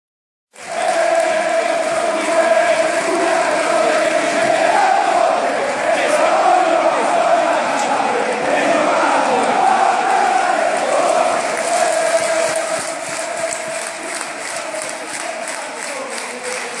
Cosenza-Reggina=2-0 Tifo Rossoblu 21 febbraio 2015
Cosenza-Reggina=2-0
Stadio San Vito
Tifo Rossoblu 21 febbraio 2015
FOTO Credits: Foto Andrea Rosito per Cosenza24.net